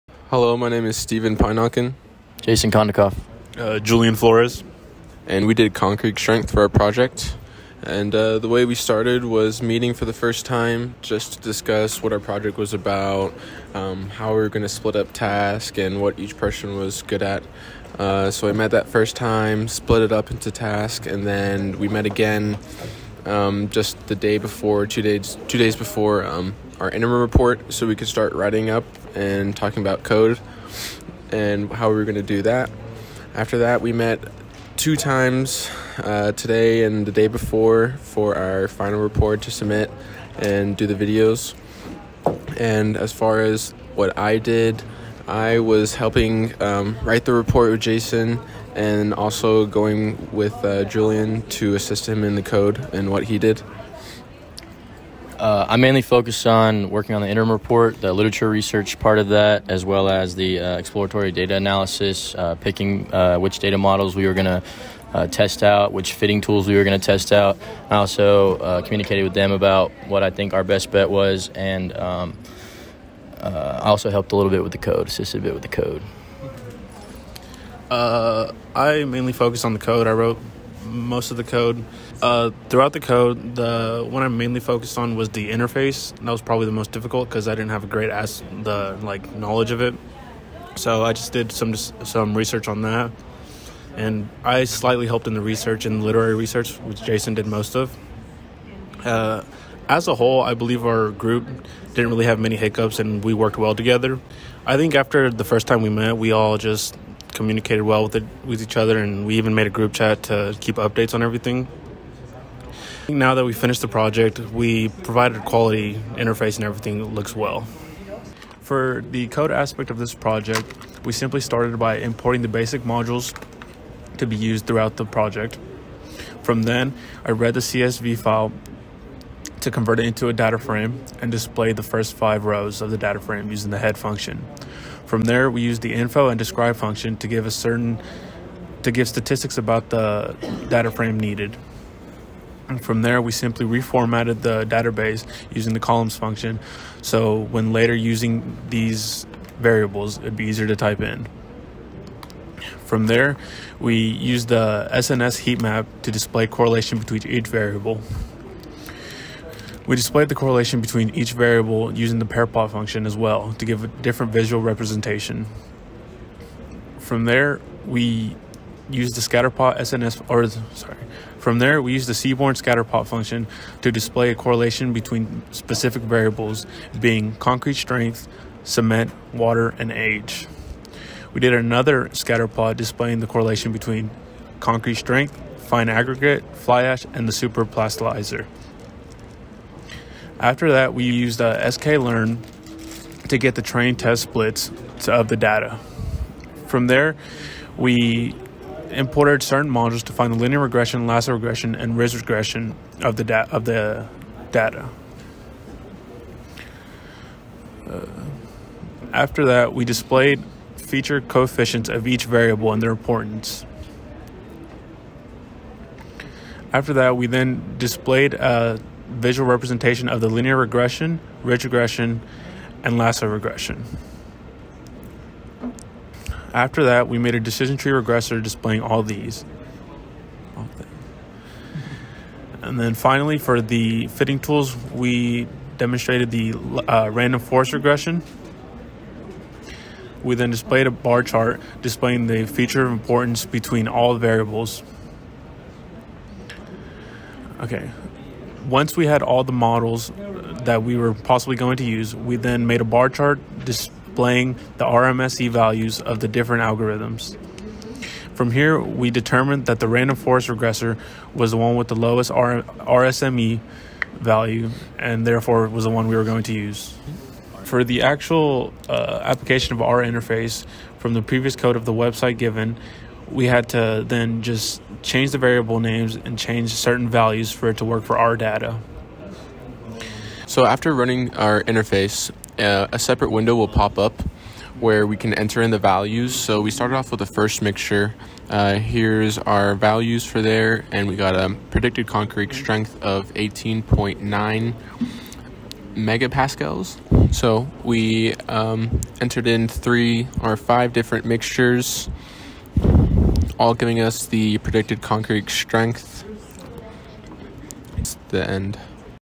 Final Project Concrete Strength
This is our Final Project video on Concrete Strength for Computational Thinking with Data Science (ENGR 1330-005).
Produced and made by:
Stephen Pijnnaken
Julian Flores
Jason Kondikoff